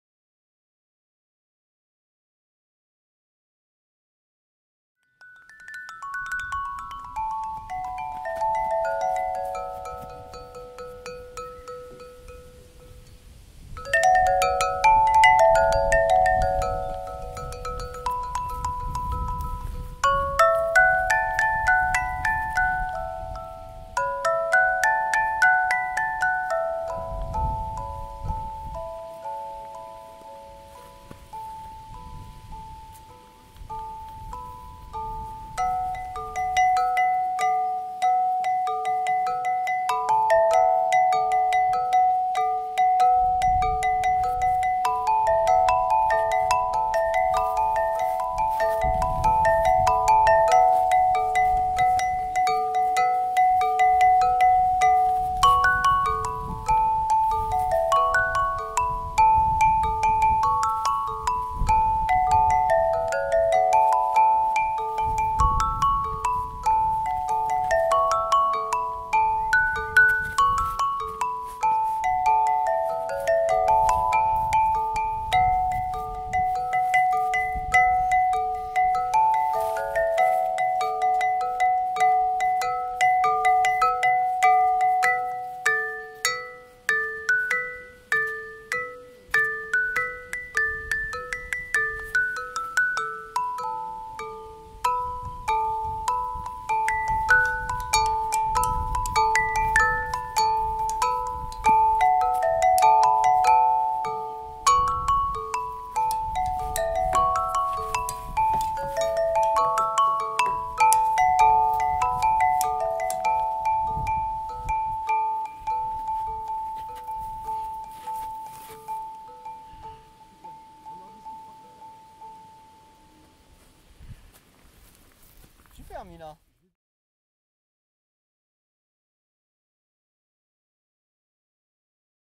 Sculpture musicale Volatil 2, ludique, interactive...
Sculpture musicale en métal de recyclage et réemploi supportant un clavier percussion en céramique créée par Atelier terre et son
Dix sculptures de ce type sont à disposition du public depuis juillet 2015 dans la prairie fleurie du château La Batie d'Urfé (42).